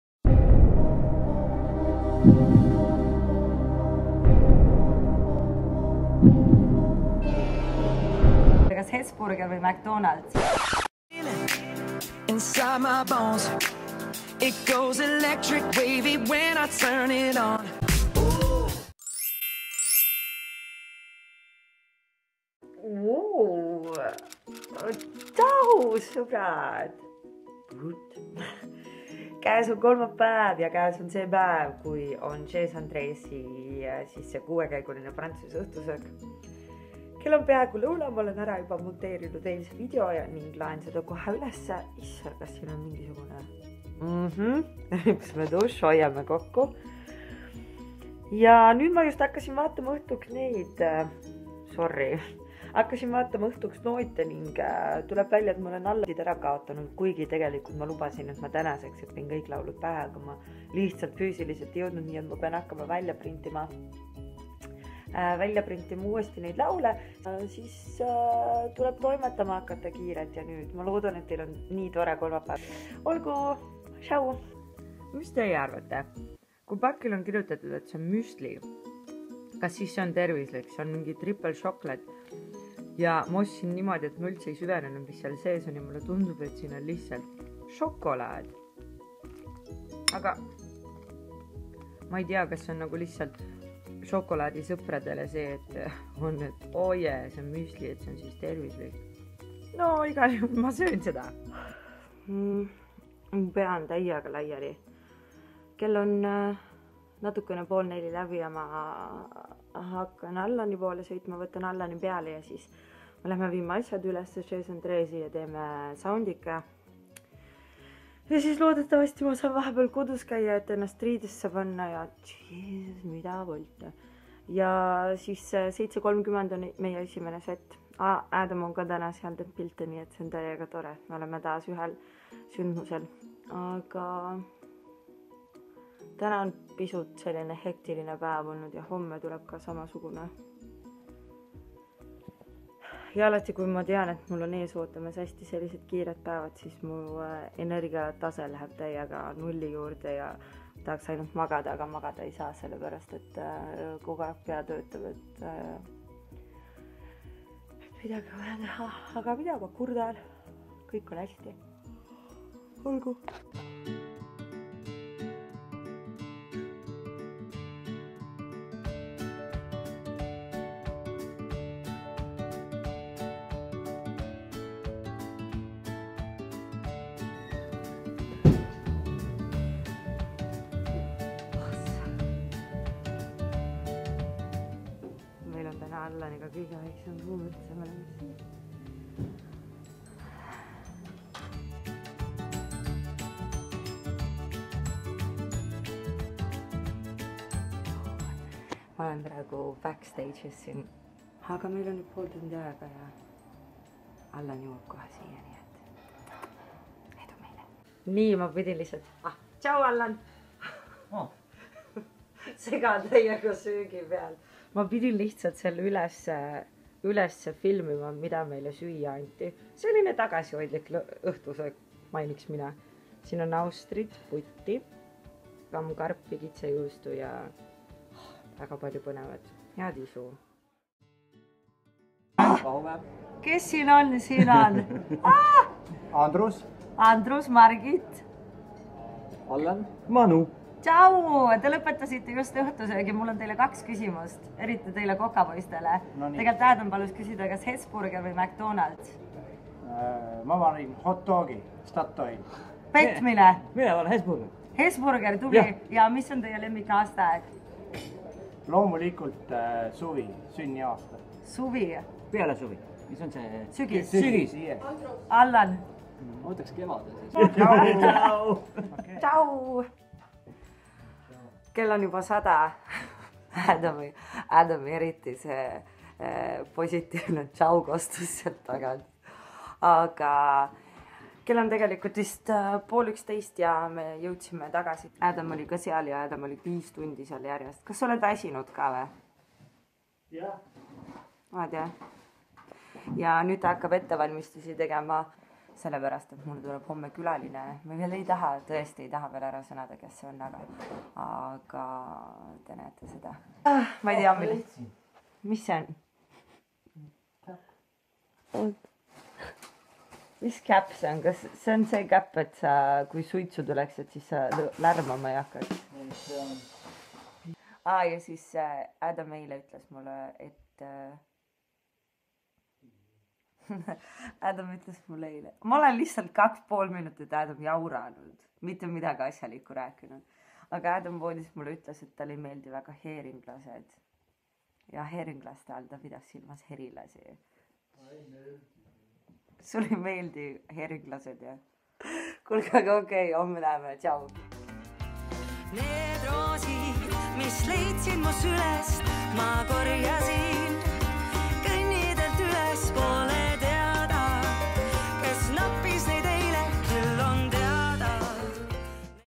PÄEV MINU ELUS #21 // COOKING WITH GOURMET CHEFS!!
Minu väljakutse 21. päev ning tänane õhtu viib meid Tartu restorani ning ka minu üllatuseks - selle kööki :)